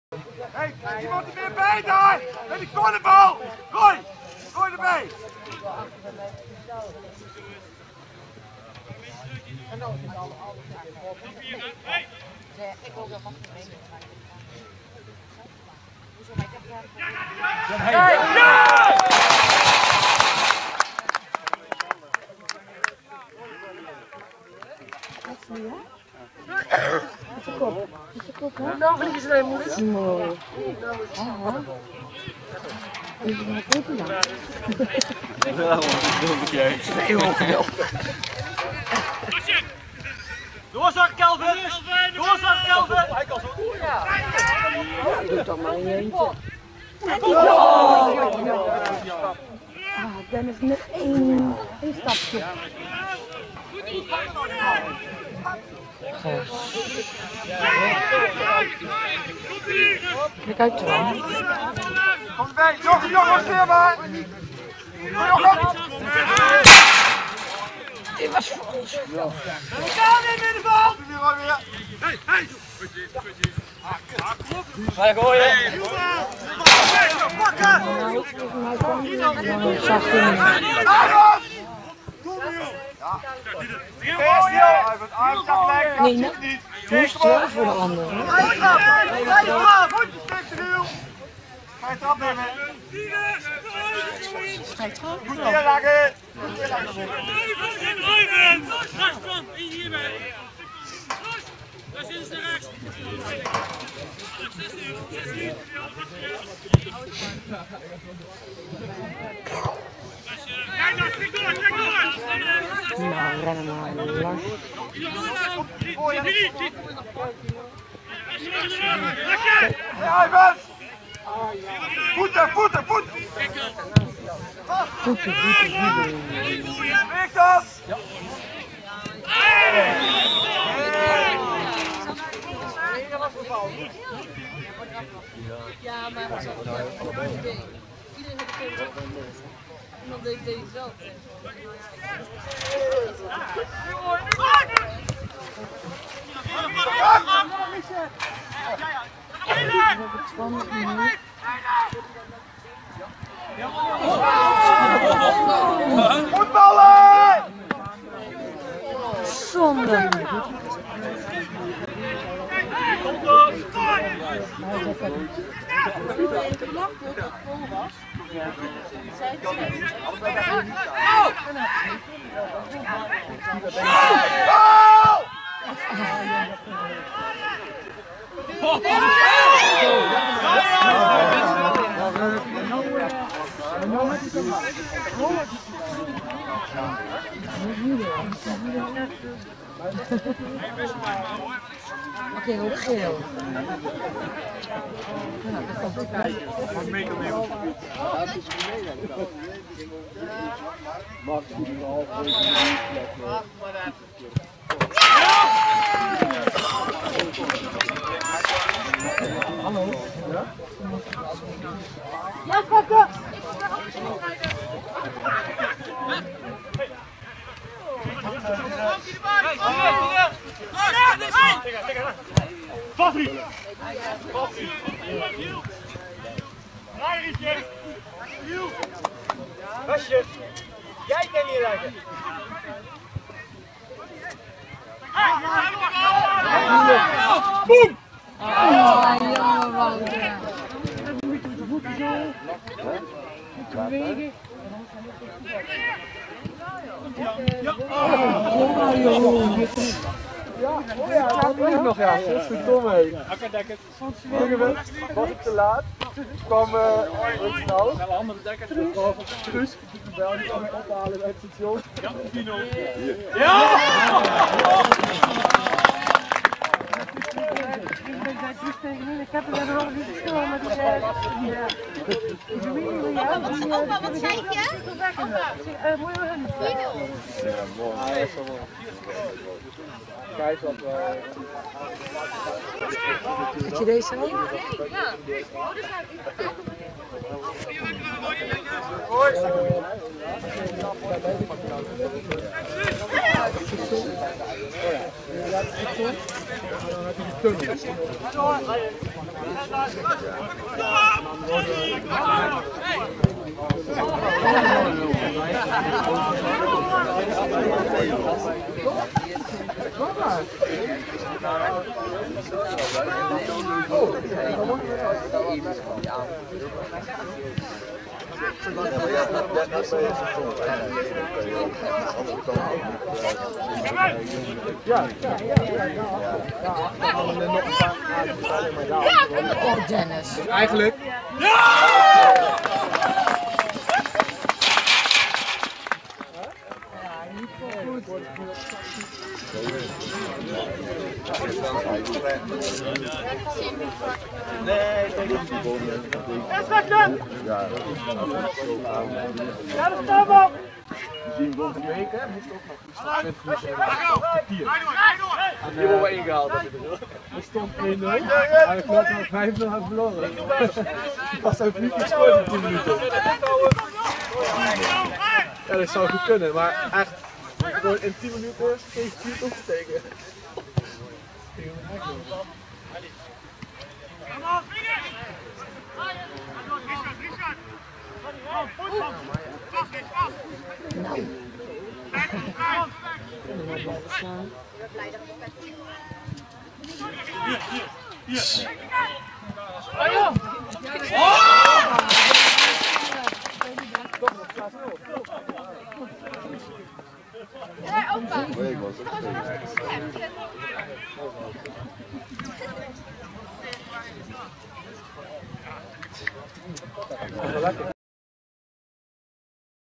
Driel 1 - SDOO 1 op 27-11-2016
ook deze derby eindigde met een ruime overwinning voor Driel: 6 - 0